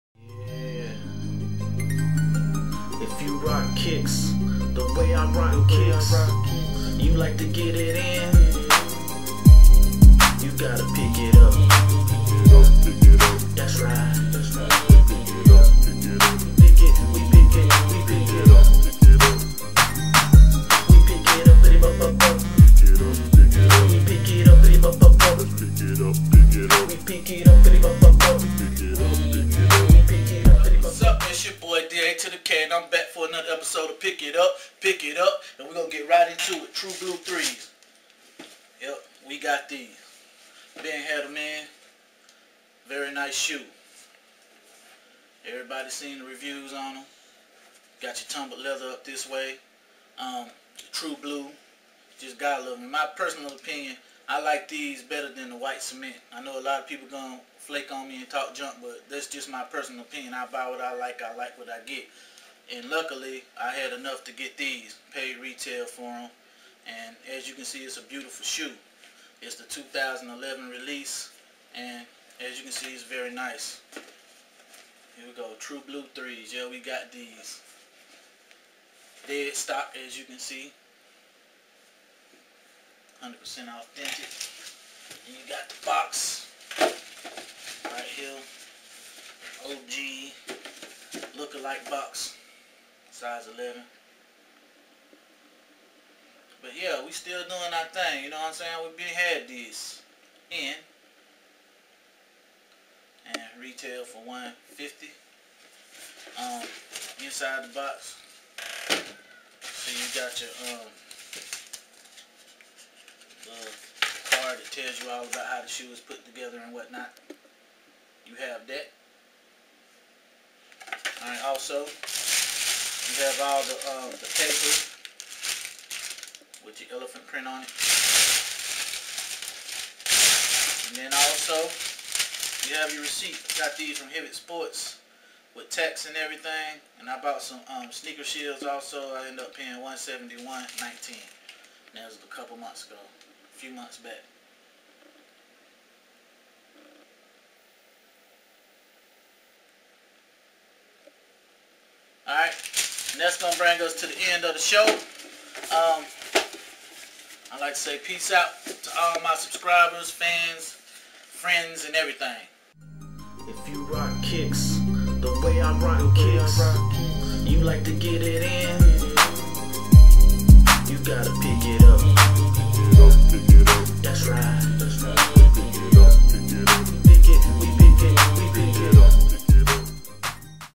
Pick It Up, Pick It Up - True Blue 3's [Episode 8]
A quick review on the 2011 True Blue 3's..You gotta see this. YUP!!